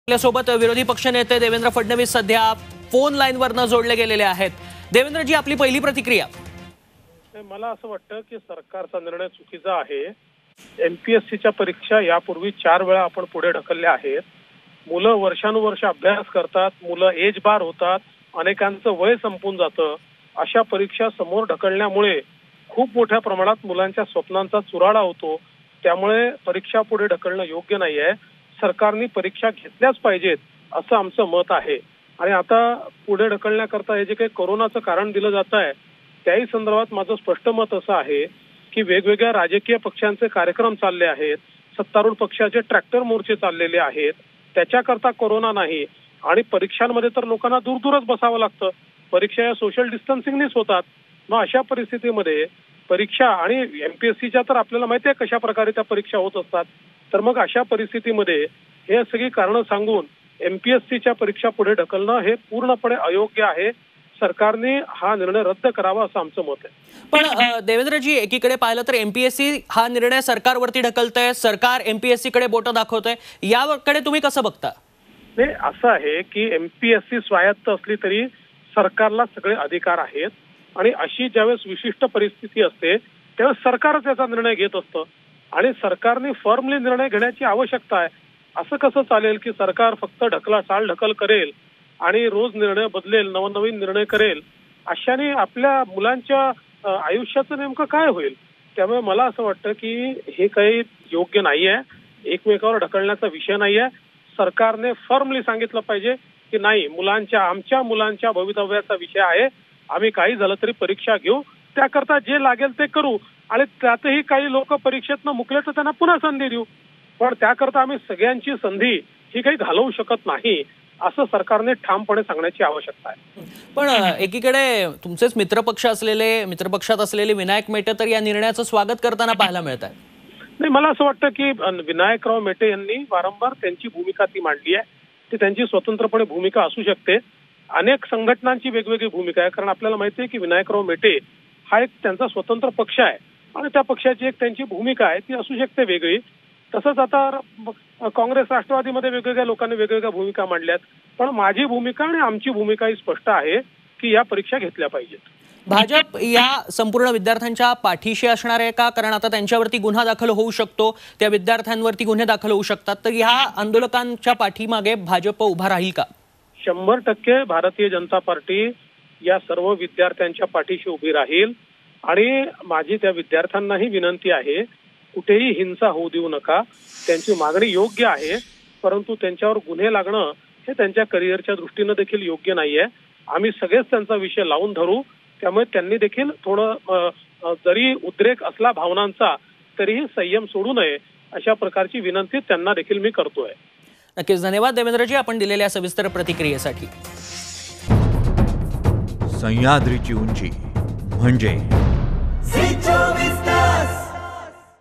MPSC Exam Postponed | विरोधी पक्ष नेते Devendra Fadnavis यांची पहिली प्रतिक्रीया
Opposition Leader | Devendra Fadnavis On Students Anger For MPSC Exam Postponed